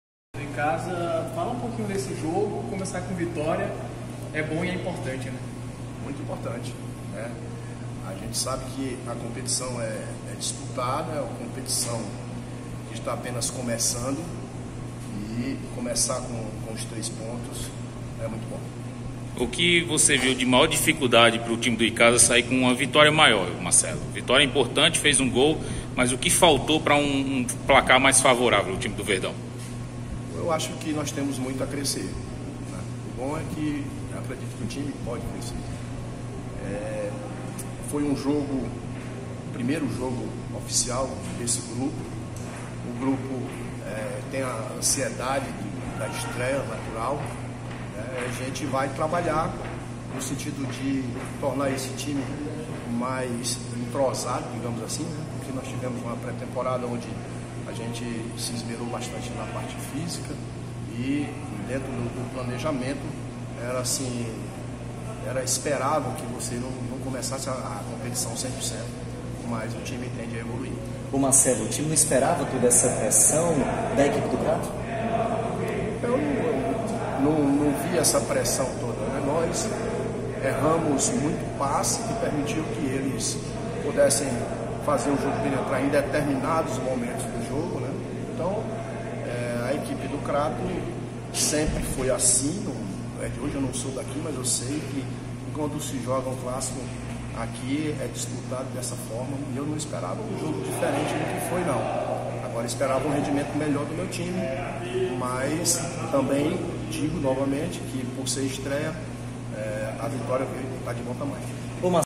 Veja o que Marcelo Villar falou depois da vitória do Icasa sobre o Crato
Crato 0x1 Icasa, 1ª rodada da Série B do Campeonato Cearense.